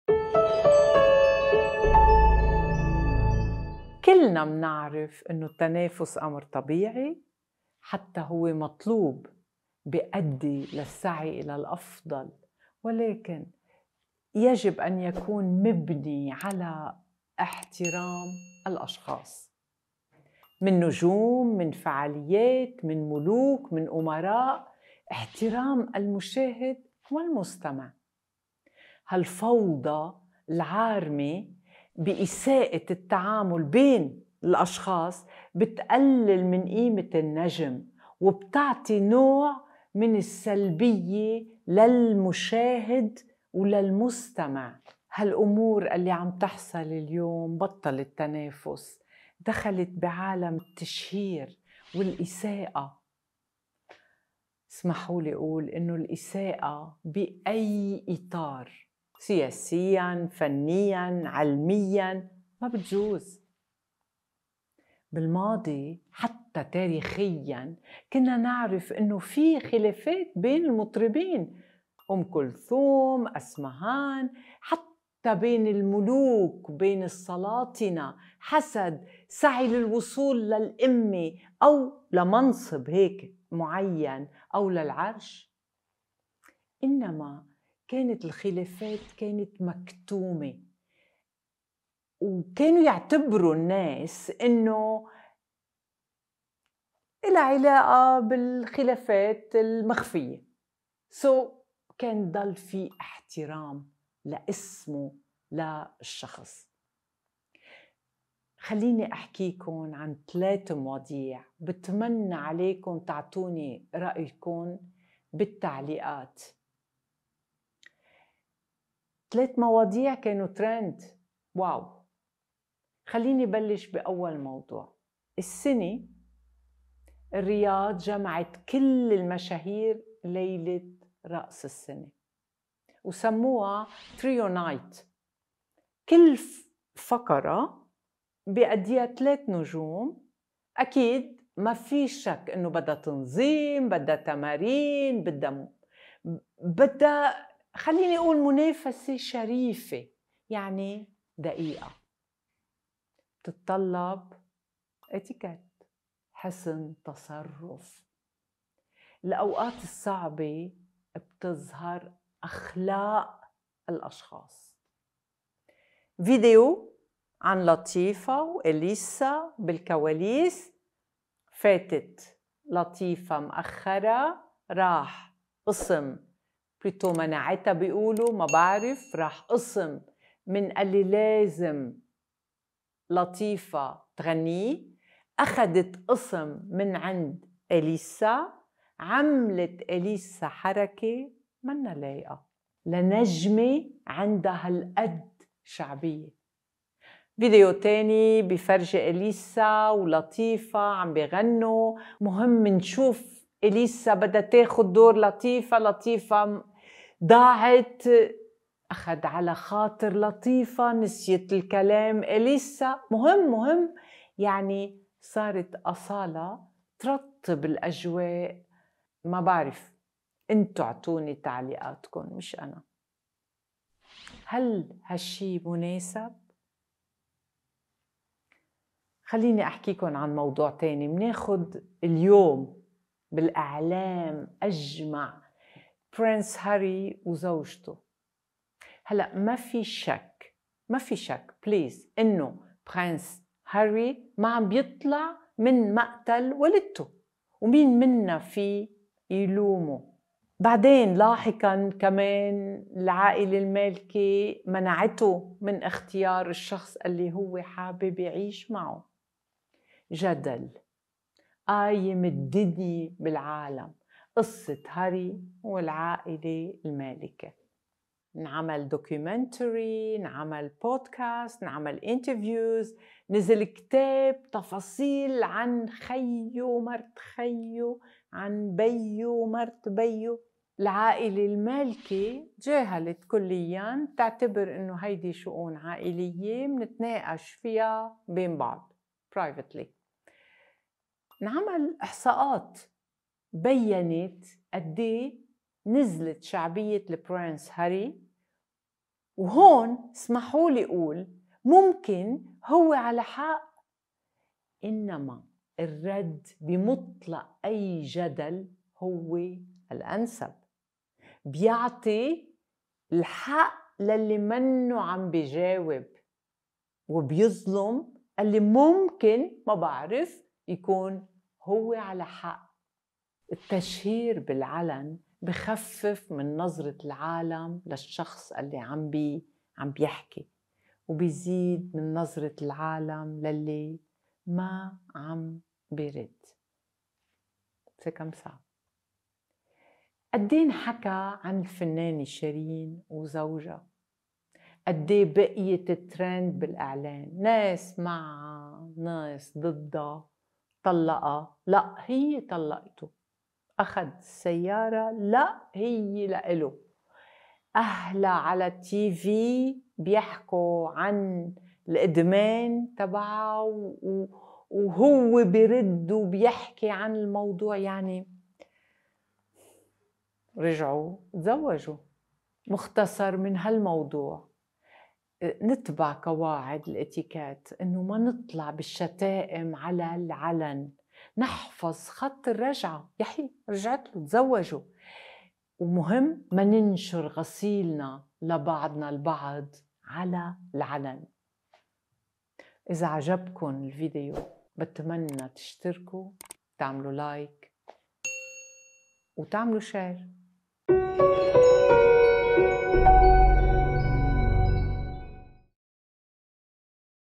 فن الاتيكيت| ٣ مواقف محرجة لفنانين ومشاهير خرقت الاتيكيت
Competition is natural and is a good thing as it forces us to do our best. However, it has to be established on the respect of others.
To celebrities, influencers, politicians, royalties, etc., the respect of their audiences is significant.
Corrupted competitions and lack of respect from well-known personalities takes away their value and provides a damaging image to viewers.
Nowadays, the different situations happening cannot be considered as competitions anymore. Everything is all about fame and judgement:
judgements in politics, in art, in education etc.
There used to be a lot of competition between esteemed artists in the past, however, no matter how intense it used to get, they never lacked
respect for each other.
The person who doesn’t speak out loud about what is unethical is at fault and could potentially oppress the one who might actually be right.
Also, By pointing fingers at others, a person decreases its standards, while increasing the others’ worth.
Those 3 trending topics educate us on important lessons about etiquette:
1- “Trio Night”, a show especially organized in Saudi Arabia on New Year’s Eve is perfect example that makes us understand how hard times
truly reveals a person's character.
2-Prince Harry and his wife' story proves how important privacy is and how easily people can be judged.
3- Sherin and her husband ‘s divorce clarifies the damages that could be done when exposing our problems on the internet.
Do not openly display your life and difficulties to the public!